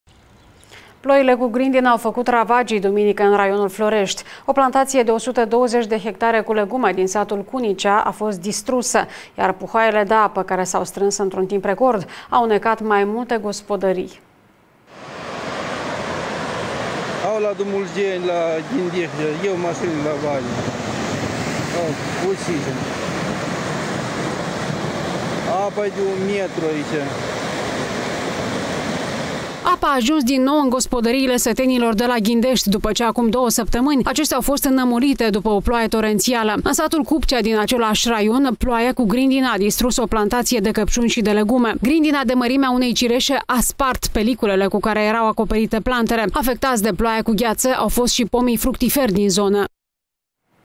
Grindina a făcut ravagii în nordul țării
Ploile cu grindină au făcut ravagii, duminică, în raionul Florești. O plantație de 120 de hectare cu legume din satul Cunicea a fost distrusă de gheață. Iar puhoaiele de apă, care s-au strâns într-un timp record, au înecat mai multe gospodării.